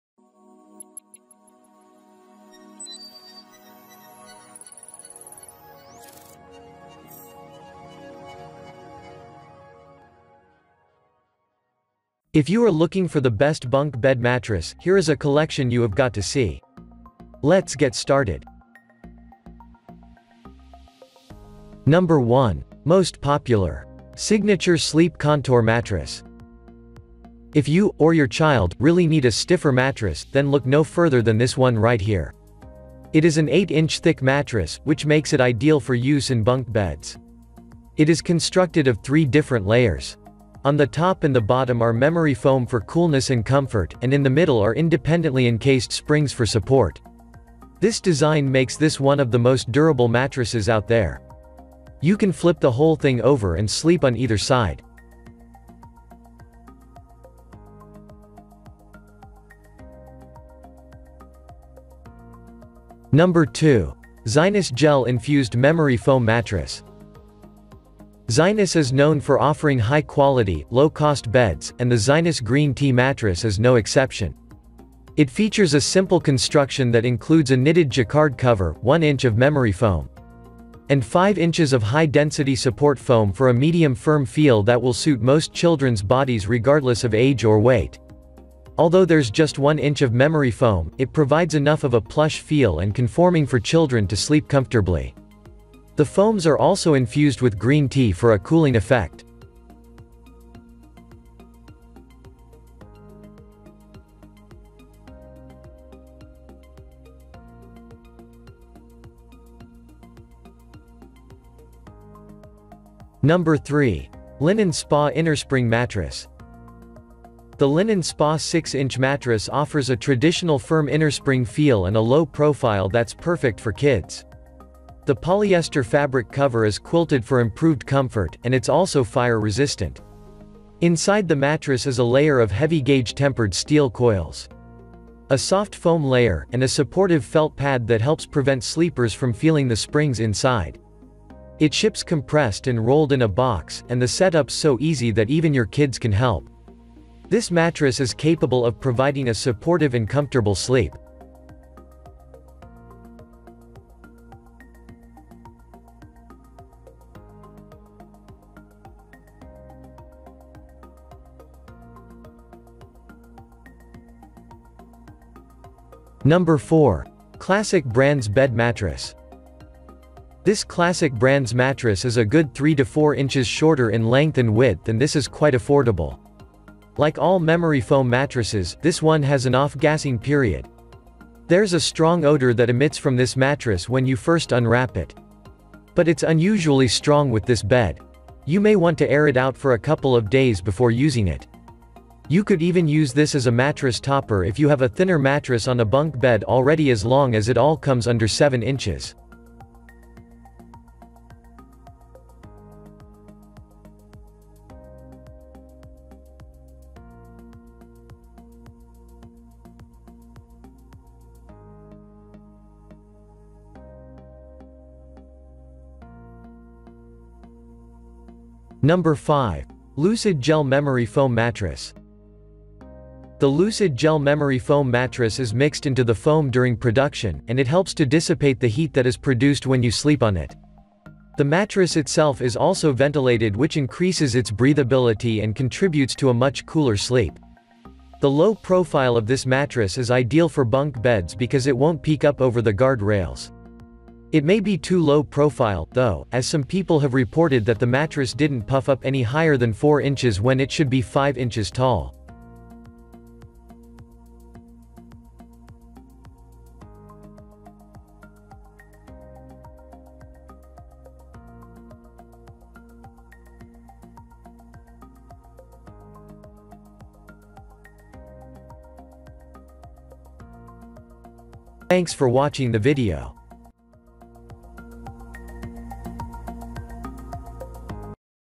Top 5 Best Bunk Bed Mattress Review in 2023
Best Bunk Bed Mattress featured in this Video:
0:21 1. Signature Sleep Contour Mattress
1:02 2. Zinus Gel-Infused Memory Foam Mattress
1:52 3. Linenspa Innerspring Mattress
2:45 4. Classic Brands Bed Mattress
3:38 5. LUCID Gel Memory Foam Mattress

What Is A Bunk Bed Mattress?
A bunk bed mattress is a regular innerspring or foam mattress which is simply thinner and smaller in size, up to 8 inches maximum, with an average of 4 to 6 inches.

What is the best Bunk Bed Mattress?
We strive to review as many products as we can, read realtime reviews and made our own ranking. We welcome all your comments.